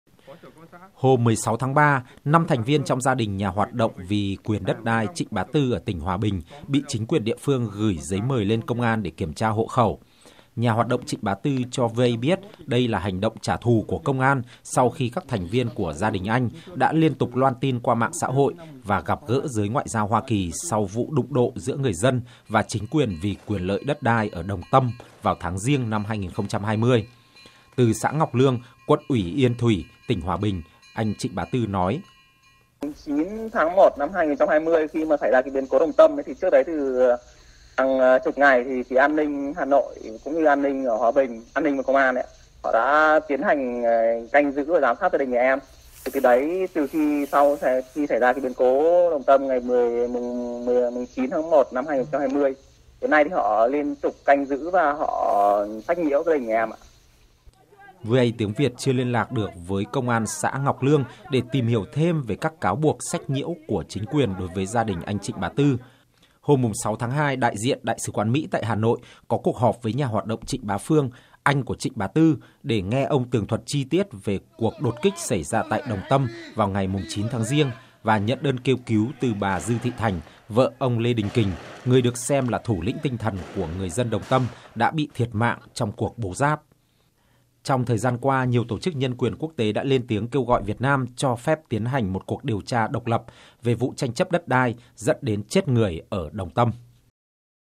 Gia đình nhà hoạt động Trịnh Bá Tư tố cáo bị chính quyền sách nhiễu (VOA)
Tin tức Nếu không vào được VOA, xin các bạn hãy vào để vượt tường lửa. Hôm 16/03, năm thành viên trong gia đình nhà hoạt động vì quyền đất đai Trịnh Bá Tư ở tỉnh Hòa Bình bị chính quyền địa phương gửi giấy mời lên công an để “kiểm tra hộ khẩu.”

Nhà hoạt động Trịnh Bá Tư cho VOA biết đây là hành động “trả thù” của công an sau khi các thành viên của gia đình anh đã liên tục loan tin qua mạng xã hội và gặp gỡ giới ngoại giao Hoa Kỳ sau vụ đụng độ giữa người dân và chính quyền vì quyền lợi đất đai ở Đồng Tâm vào tháng 1/2020.

Từ xã Ngọc Lương, quận Yên Thủy, tỉnh Hòa Bình, anh Trịnh Bá Tư, nói:

“Trước và sau khi diễn ra biến cố Đồng Tâm hôm 09/01 thì an ninh ở Hà Nội và Hòa Bình đã tiến hành canh giữ và giám sát gia đình tôi. Từ 09/01 thì họ liên tục sách nhiễu gia đình tôi.”

VOA Tiếng Việt chưa liên lạc được với công an xã Ngọc Lương để tìm hiểu thêm về các cáo buộc sách nhiễu của chính quyền đối với gia đình anh Trịnh Bá Tư.

Anh Trịnh Bá Tư cho VOA biết:

“Sáng nay thì họ gửi giấy thông báo mời 5 gia đình nhà tôi đến trụ sở công an xã Ngọc Lương để làm việc. Tuy nhiên gia đình tôi đã từ chối.

Đoạn video mà gia đình anh Trịnh Bá Tư ghi lại trên trang Facebook cho thấy các công viên đọc giấy mời, đọc biên bản và họ ra về trong ôn hòa.

Hôm 6/2, đại diện của Đại sứ quán Mỹ tại Hà Nội có cuộc họp với nhà hoạt động Trịnh Bá Phương, anh của anh Trịnh Bá Tư, để nghe tường thuật chi tiết về cuộc đột kích xảy ra tại Đồng Tâm vào ngày 09/01 và nhận đơn kêu cứu từ bà Dư Thị Thành, vợ ông Lê Đình Kình – người được xem “thủ lĩnh tinh thần” của người dân Đồng Tâm đã bị thiệt mạng trong cuộc bố ráp.

Vụ đột kích đã khiến cho ít nhất 4 người thiệt mạng, bao gồm ông Lê Đình Kình và 3 công an thiệt mạng.

Anh Trịnh Bá Tư cho biết thêm:

“Trước đó họ có nhiều hành động khiêu khích đánh đập và bắt giữ anh Trịnh Bá Phương và cho côn đồ đi theo tôi.

“Mục đích của họ là gài bẫy để bắt giữ người tranh đấu. Họ có thể dàn dựng để bắt giữ những người trong gia đình tôi.”

Sau vụ đột kích, Công an Hà Nội ngày 13/01 ra quyết định khởi tố 22 người dân Đồng Tâm đã bị bắt giam, theo sau quyết định khởi tố của Bộ Công an đối với họ về 3 tội danh “giết người; tàng trữ, sử dụng vũ khí trái phép; và chống người thi hành công vụ”.

Trong thời gian qua, nhiều tổ chức nhân quyền quốc tế đã lên tiếng kêu gọi Việt Nam cho phép tiến hành một cuộc điều tra độc về vụ tranh chấp đất đai dẫn đến chết người ở Đồng Tâm.